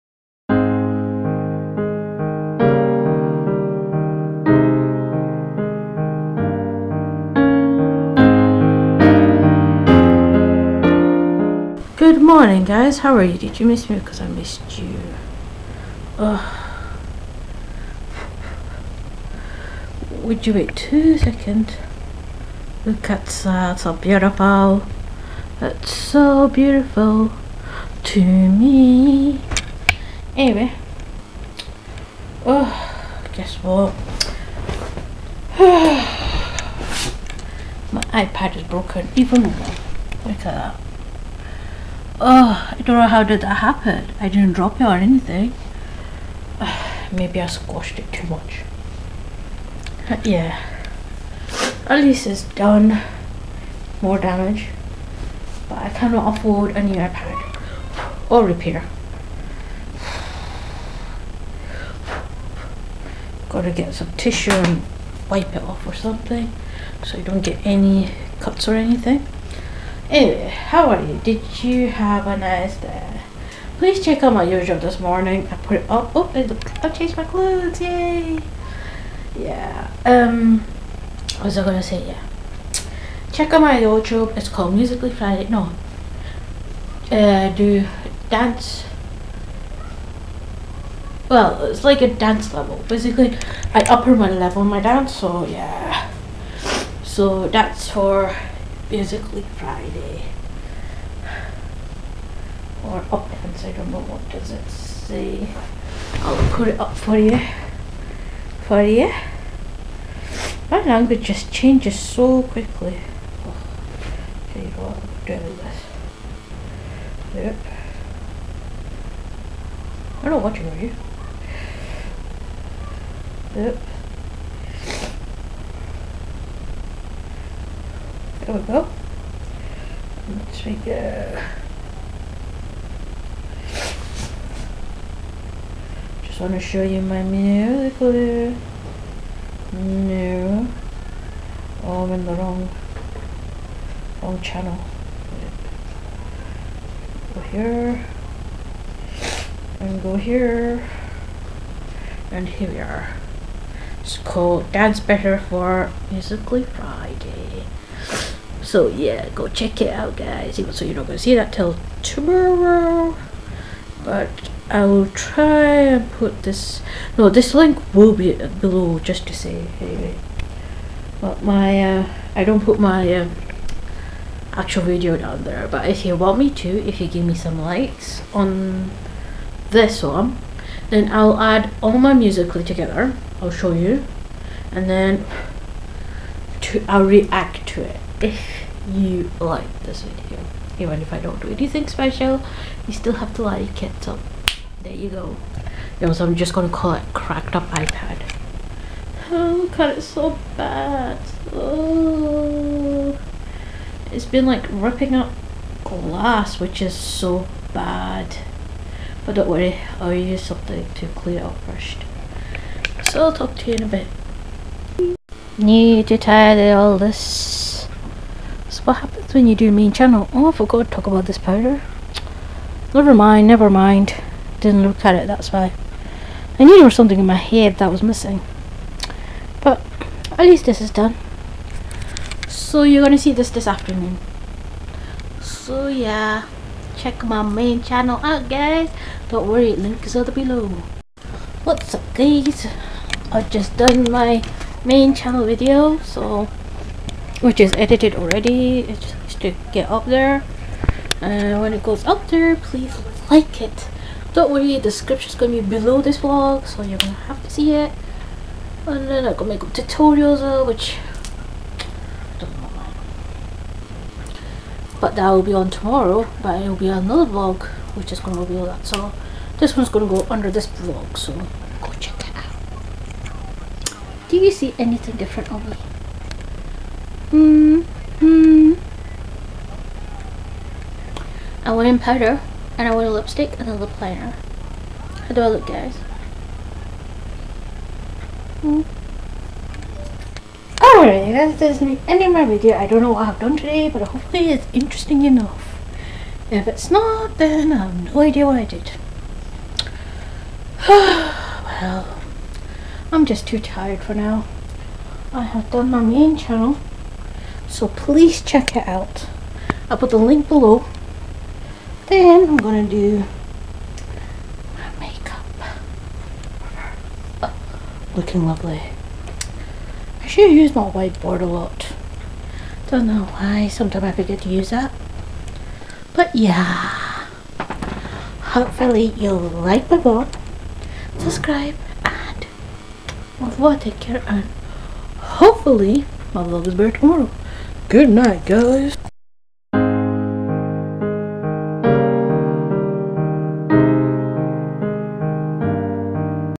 Broken iPad & Editing Main Channel
06.05.17

I woke up in the morning, first thing I see is my iPad screen is even more cracked. I am thinking how did this happen! I might of squashed it or dropped it down.

I started to get ready for the Main Channel video, which will be linked below.

MAKEUP

Maybelline New York Sand Powder
MAC Soar Lip Liner
MAC Fresh Brew Lipstick